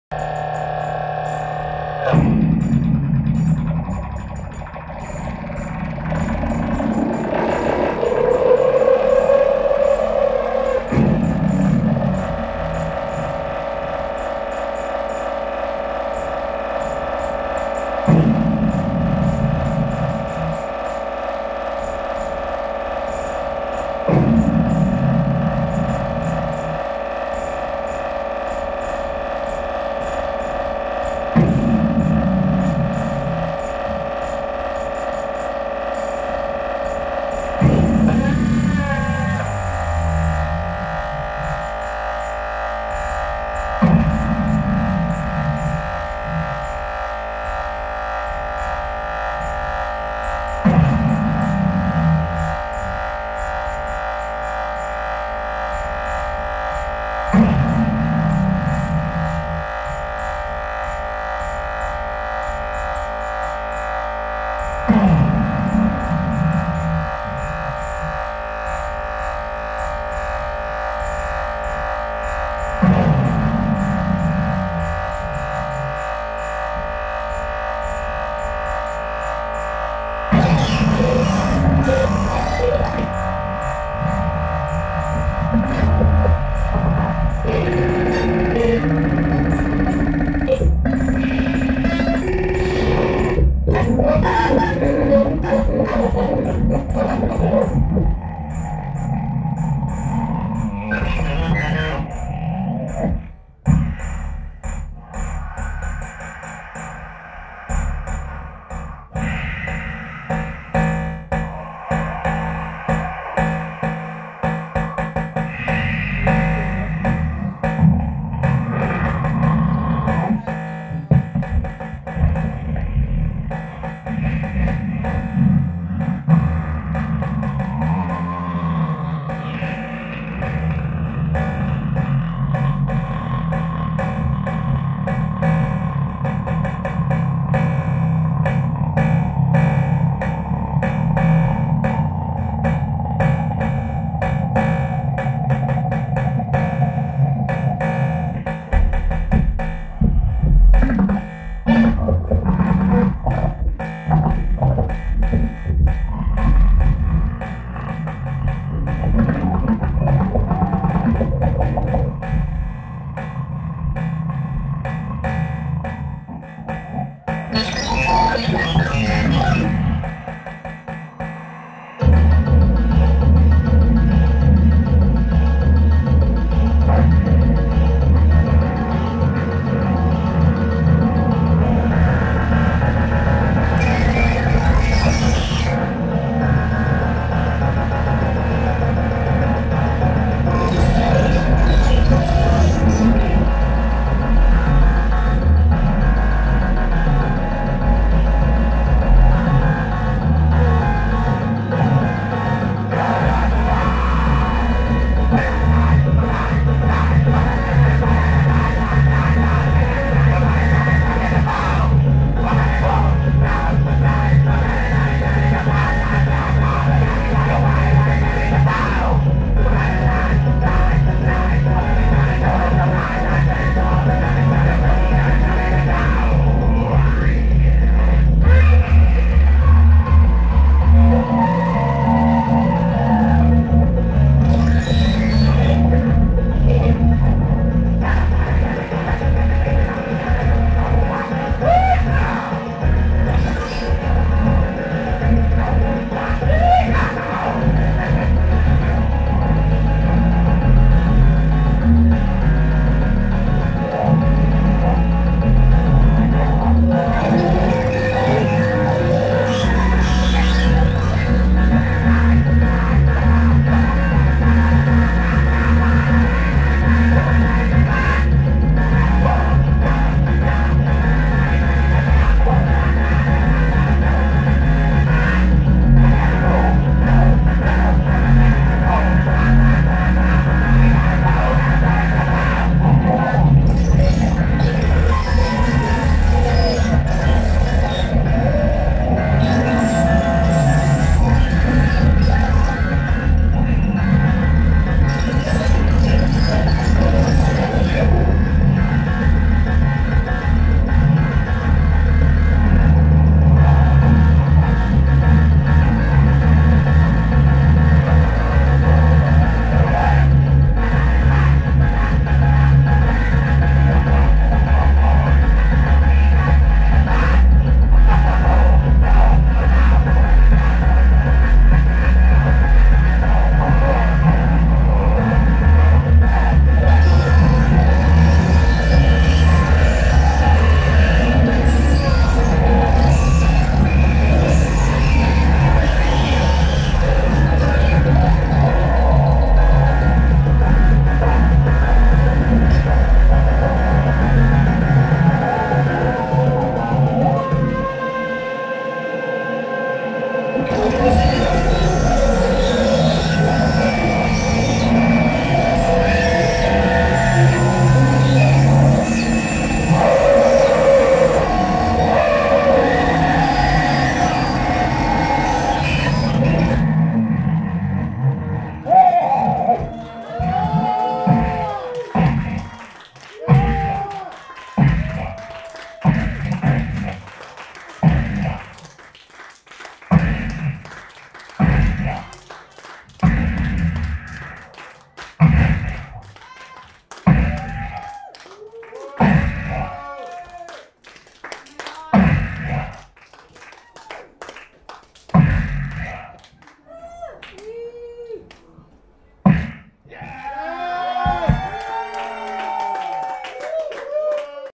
bran(...)pos Live at MOCO 2014
excerpt of bran(...)pos set at MOCO gallery in Oakland 4/19/14